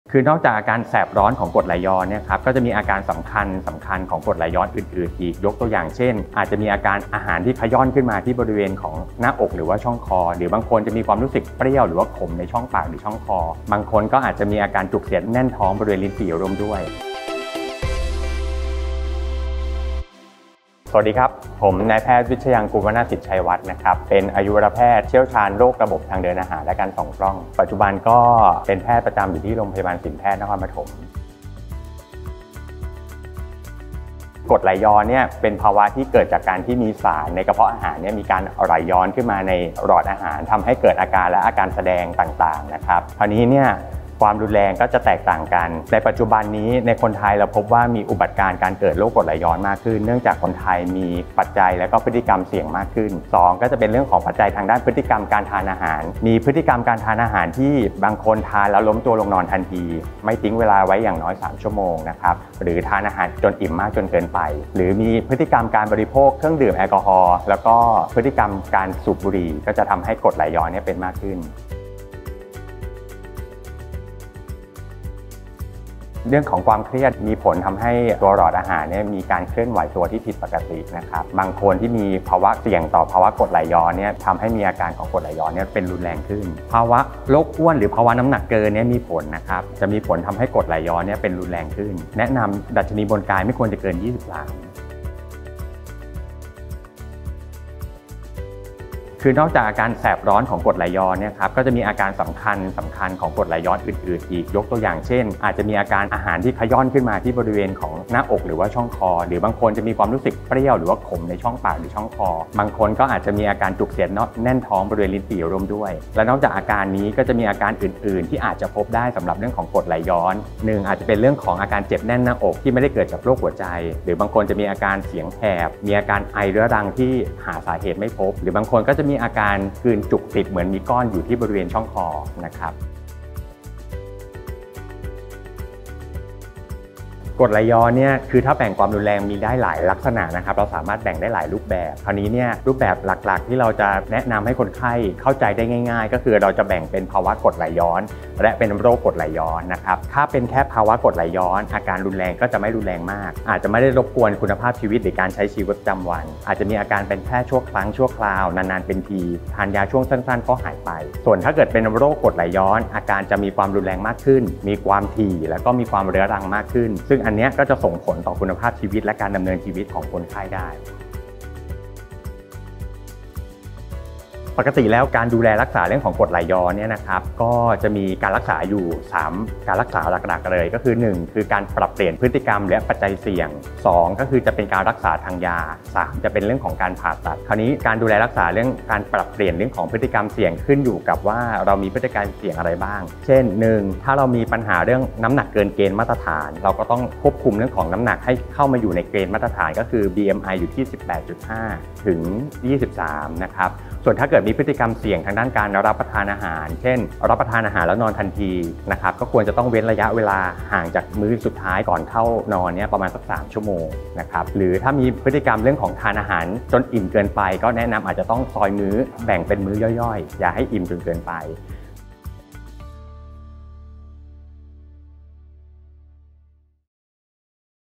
หมอขอแชร์ SS2 EP.15 : แสบร้อนกลางอกอาการแบบไหน เรียกกรดไหลย้อน ? | รพ.สินแพทย์ นครปฐม
“กรดไหลย้อน” แค่แสบอกอย่างเดียวบอกได้ไหม
หรือมีอาการอื่น ๆ ของโรคที่สังเกตได้ร่วมด้วย
หมอขอแชร์ EP นี้ คุณหมอ วิชญางกูร วนาสิทธชัยวัฒน์
อายุรแพทย์ รพ.สินแพทย์ นครปฐม จะมาไขข้อสงสัย
เกี่ยวกับโรคกรดไหลย้อนให้เราฟังกัน 🧑🏻‍⚕️

🔍 อยากรู้เรื่องไหน คลิกเลย!
00:32 - 01:12 อะไรเป็นสาเหตุที่ทำให้คนเป็นกรดไหลย้อนกันมากขึ้น ?
01:13 - 01:35 ความเครียด และโรคอ้วน มีผลทำให้เกิดโรคกรดไหลย้อนหรือไม่ ?
01:36 - 02:13 นอกจากอาการแสบร้อน มีอาการใดอีกบ้าง ที่บ่งบอกว่าเป็นกรดไหลย้อน ?
02:14 - 02:51 ฏรคกรดไหลย้อนมีความรุนแรงทั้งหมดกี่ระดับ ?
02:52 - 03:41 โรคกรดไหลย้อนมีวิธีการรักษาอย่างไรบ้าง ?

🤔 ติดตามประเด็นสุขภาพอื่น ๆ ได้ที่ 👉🏻